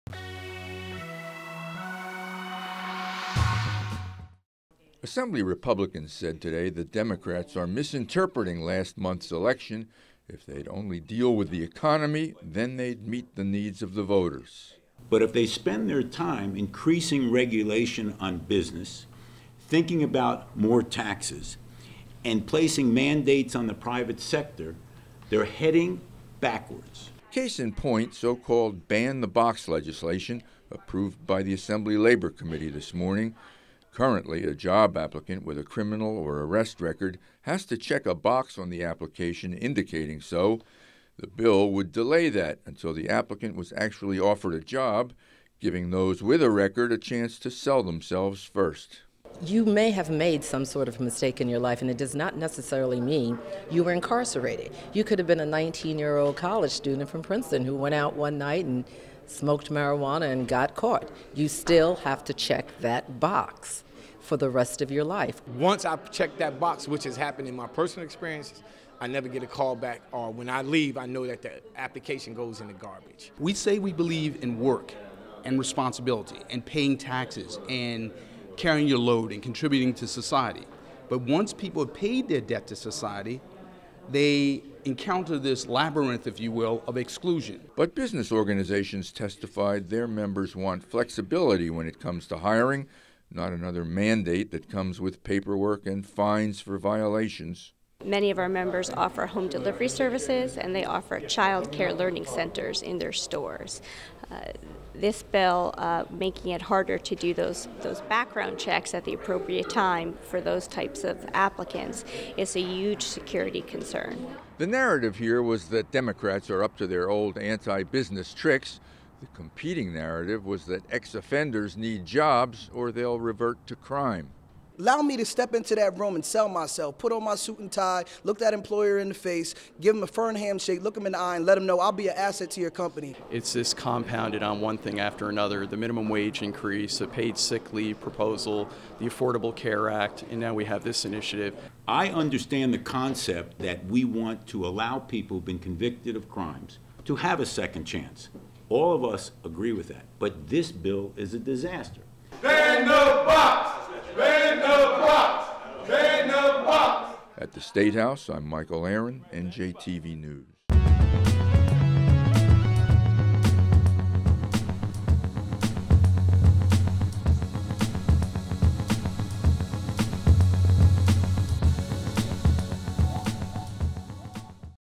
Lawmakers Consider 'Ban the Box' Legislation
Ban the Box legislation would delay a job applicant's disclosure of a criminal record until after an offer has been made. For more New Jersey news, visit NJTV News